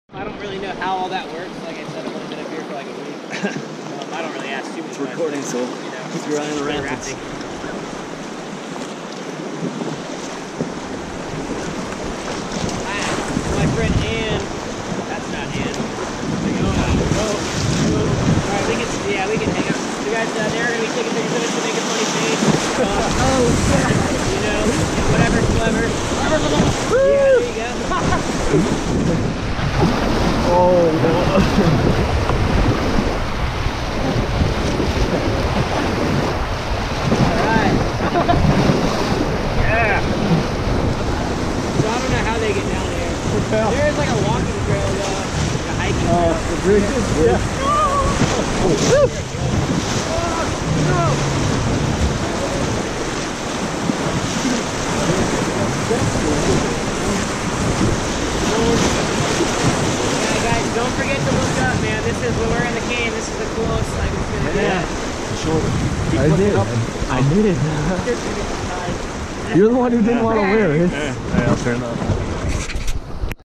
Testing the GoPro
Our first little bumpy rapid as we start heading down the upper Colorado.  July 2018